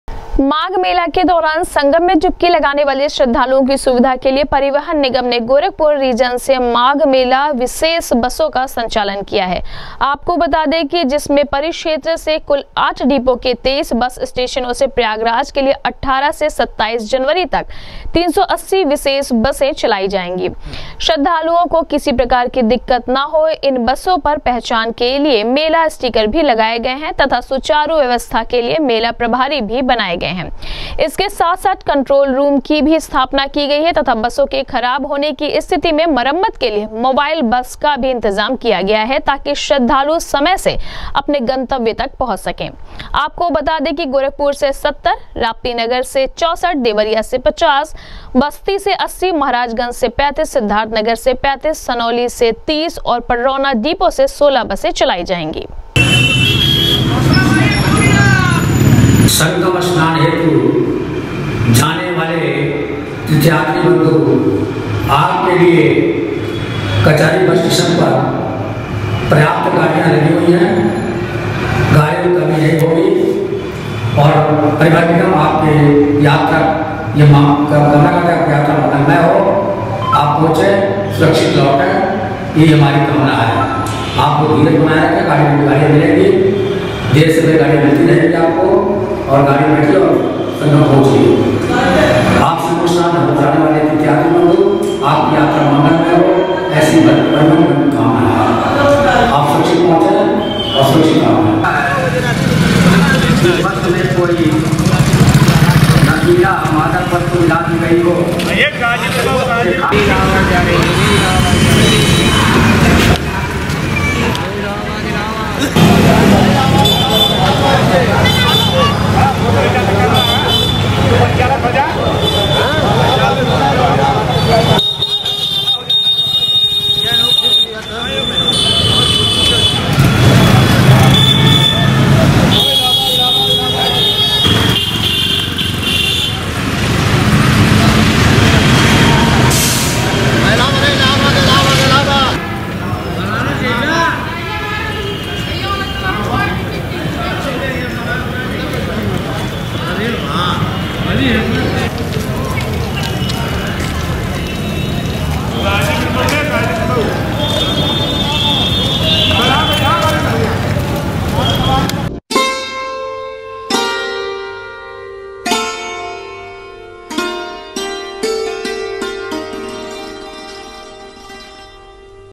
माघ मेला के दौरान संगम में डुबकी लगाने वाले श्रद्धालुओं की सुविधा के लिए परिवहन निगम ने भी कसी कमर
माघ मेला के दौरान संगम में डुबकी लगाने वाले श्रद्धालुओं की सुविधा के लिए परिवहन निगम ने भी कसी कमर, 27 जनवरी तक चलाई जाएंगी 380 विशेष बसें